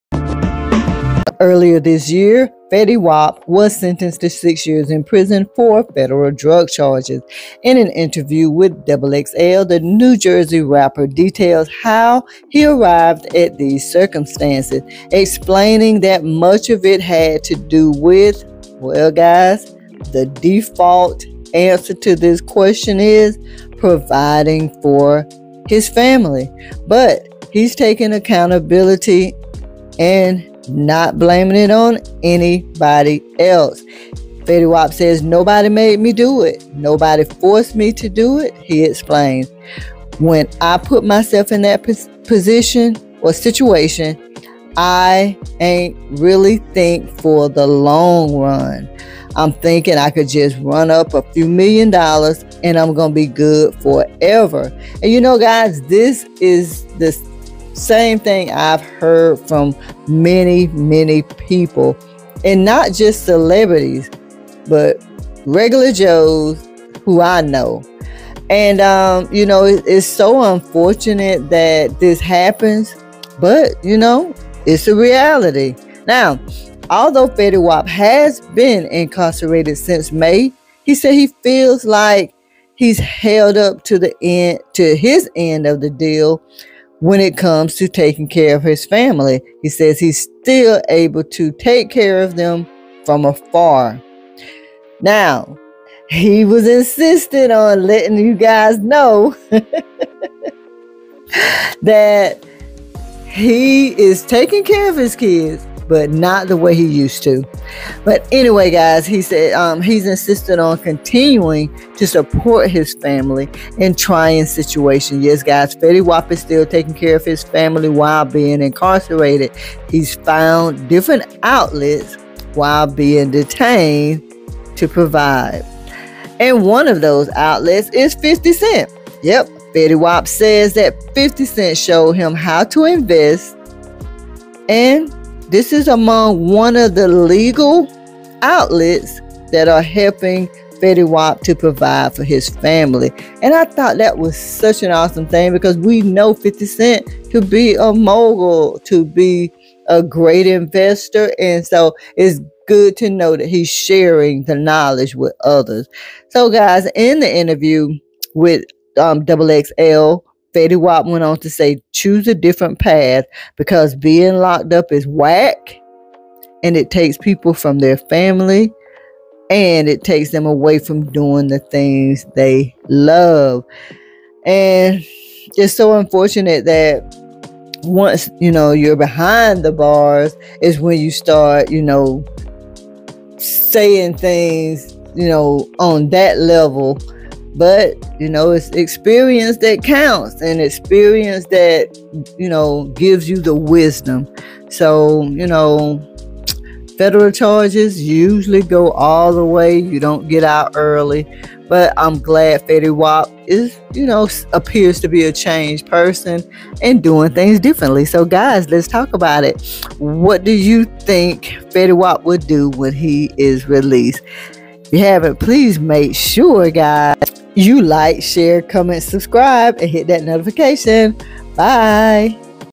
50 Cent helped Fetty Wap make money differently
Fetty Wap is taking full accountability for his actions and days 50 Cent helped him make money differently.

Nobody made me do it. Nobody forced me to do it,” he explains. “When I put myself in that situation, I ain’t really think for the long run. I’m thinking I could just run up a few million dollars, and I’m gon’ be good forever.”

Video Credit:
Getty
Photo Credit:
Yahoo
Red Ferns
Theo Wargo